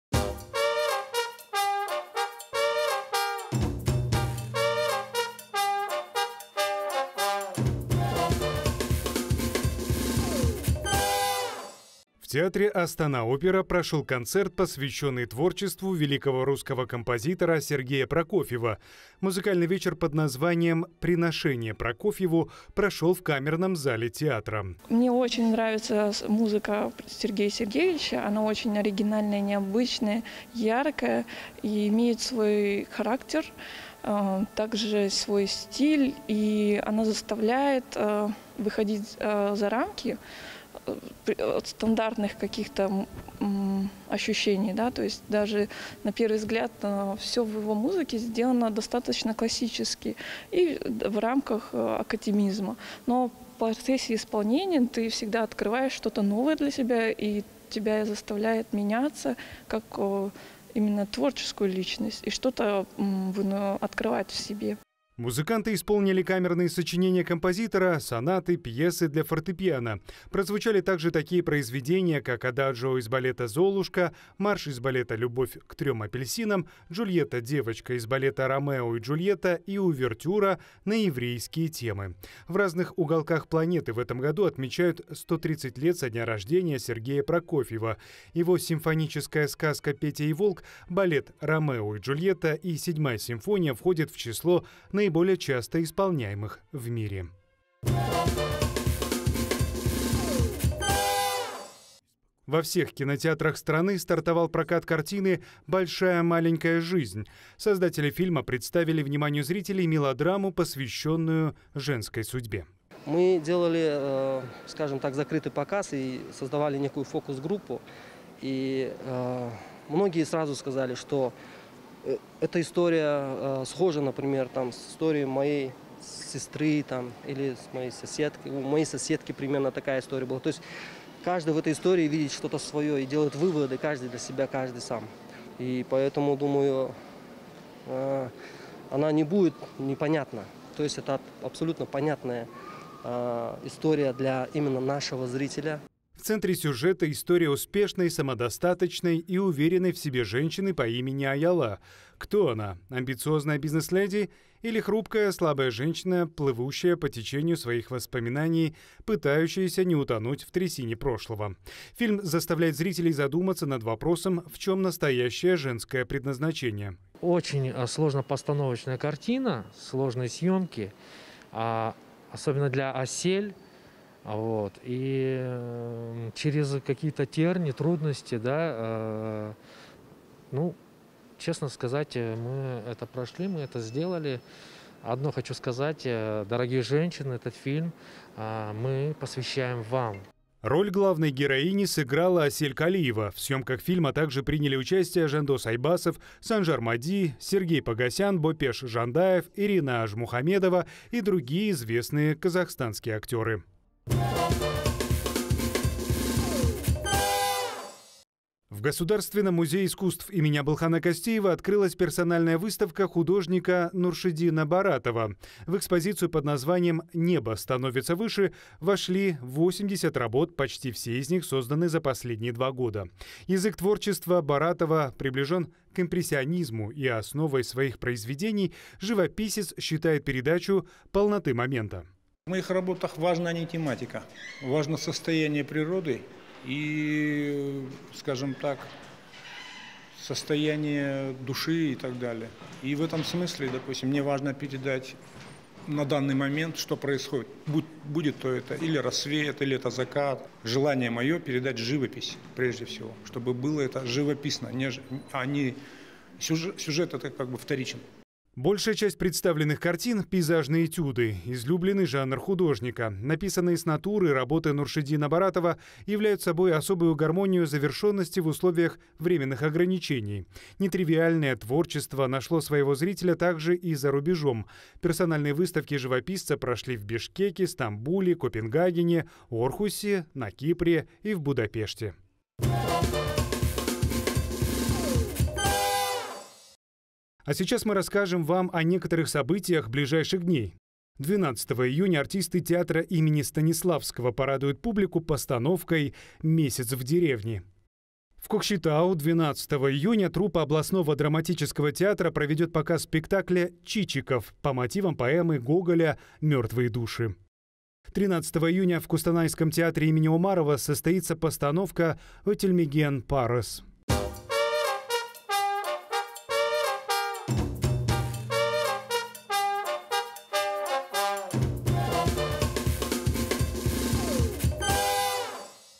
Персональная выставка художника Нуршидина Баратова | Культвояж
Новости этого выпуска:

- В театре «Астана Опера» прошёл концерт, посвящённый творчеству великого русского композитора Сергея Прокофьева;

- Во всех кинотеатрах страны стартовал прокат кинокартины «Большая маленькая жизнь». Создатели фильма представили вниманию зрителей мелодраму, посвященную женской судьбе;

- В Алматы, в Государственном музее искусств имени Абылхана Кастеева, открылась персональная выставка художника Нуршидина Баратова. В экспозицию под названием «Небо становится выше» вошли 80 работ.